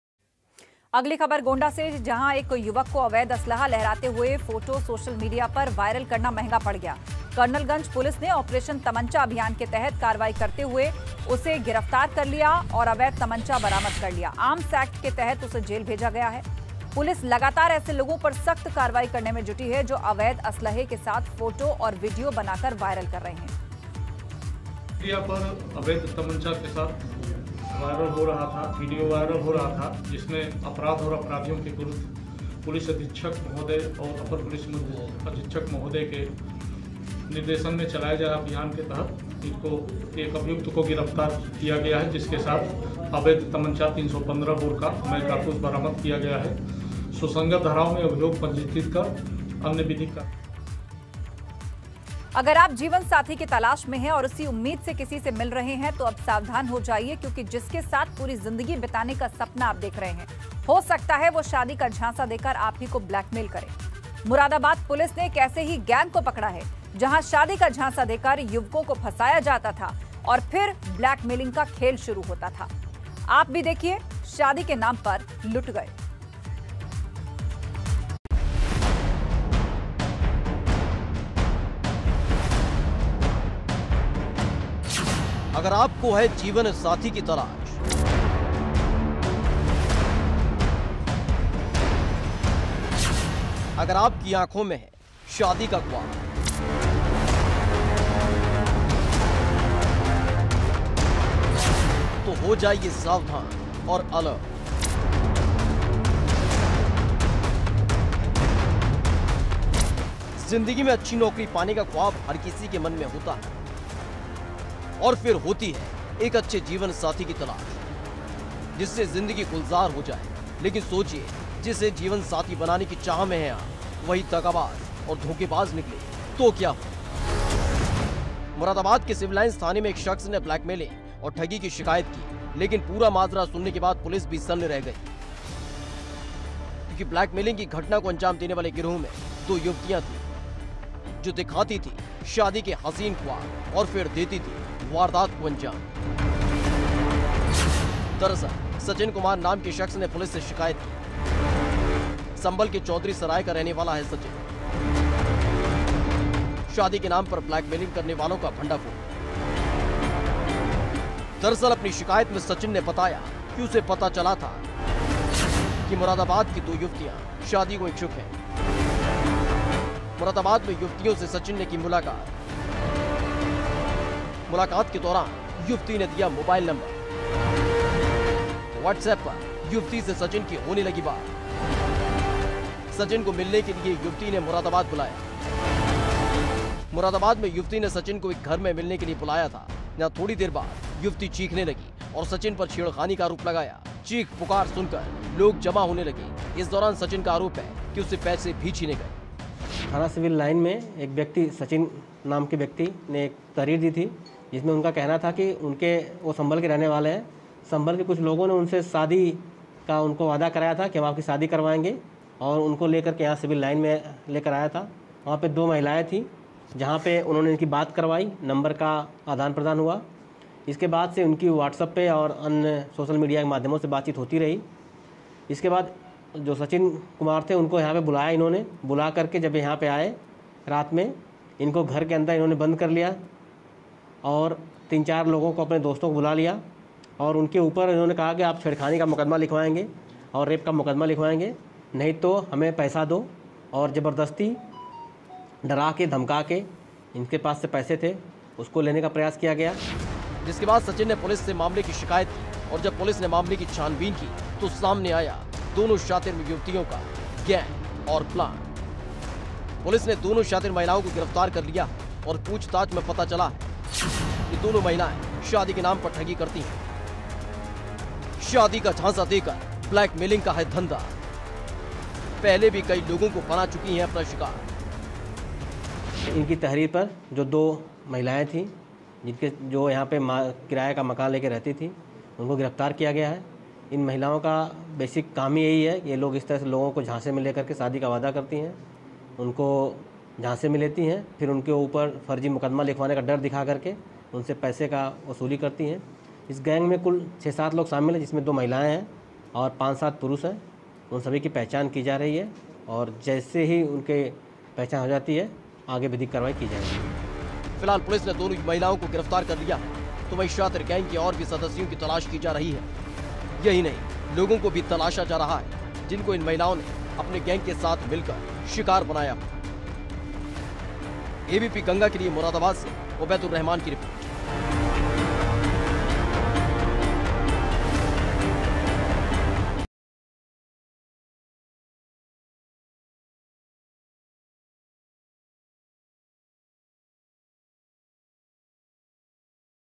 Gonda में अवैध असलहा लहराना युवक पर पड़ा महंगा, आर्म्स एक्ट के तहत हुई गिरफ्तारी | ABP Ganga
गोंडा में अवैध असलहा लहराना एक युवक पर महंगा पड़ गया। दरअसल उसने असलहा लहराते हुए फोटो सोशल मीडिया पर डाल दिया जिसके बाद आरोपी युवक को गिरफ्तार कर लिया गया और आर्म्स एक्ट के तहत उसे जेल भेजा गया। .

ABP Ganga